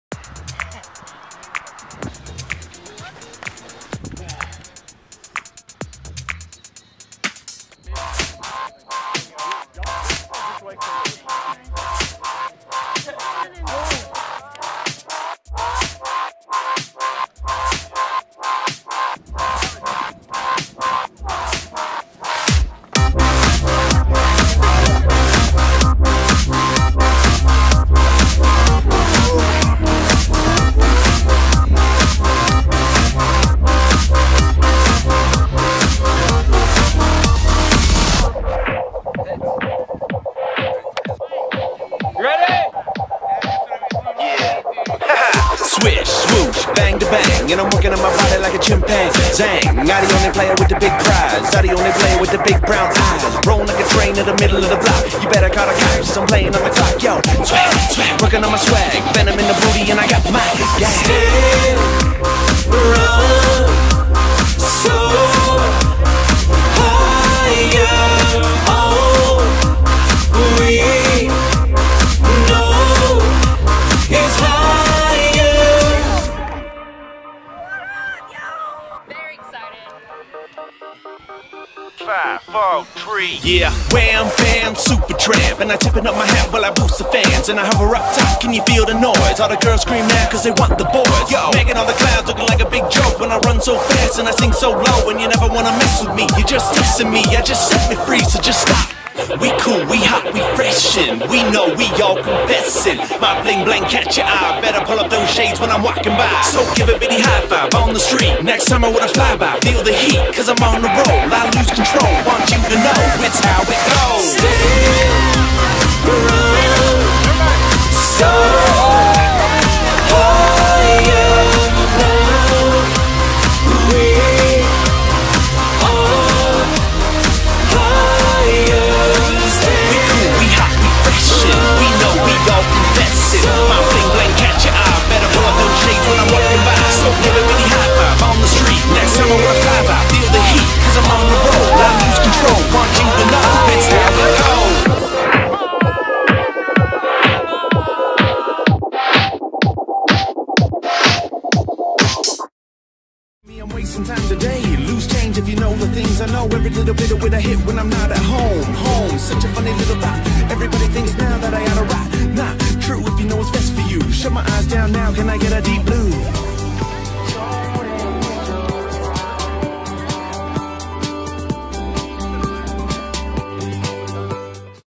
Frisbee for Life - The Ultimate Group
This young group of fun, athletic people decided to make Ultimate Frisbee not something they want to do, but something they have to do every single week. Their passion for the sport has grown over time and invite new people every week to come join and have a great experience!

Song made by Scott & Brendo, these guys are truly talented, check them out!

Big Thanks to the frisbee group for letting me come out and shoot them, had a blast! :)   Also big thanks to Drake Amador for coming out to help me on the last day!